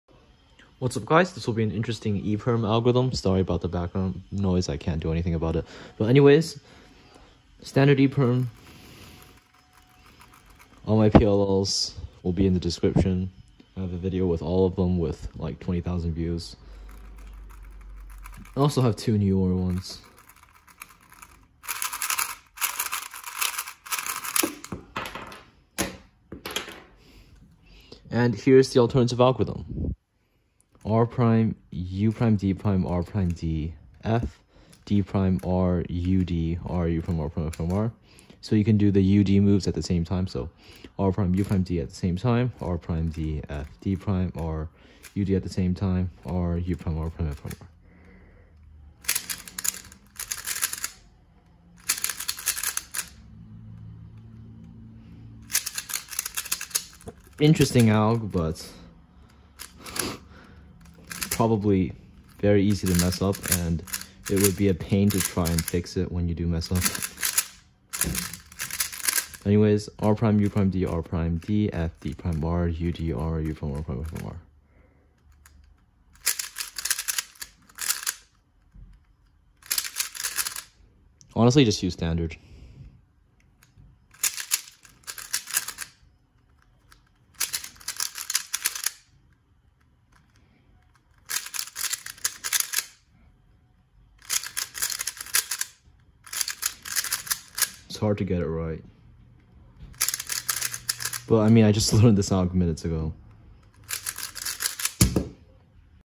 Interesting E Perm Algorithm
Thanks to sequesteredcuber for the alg

R' U' D' R' D F D' R U D R U' R' F' R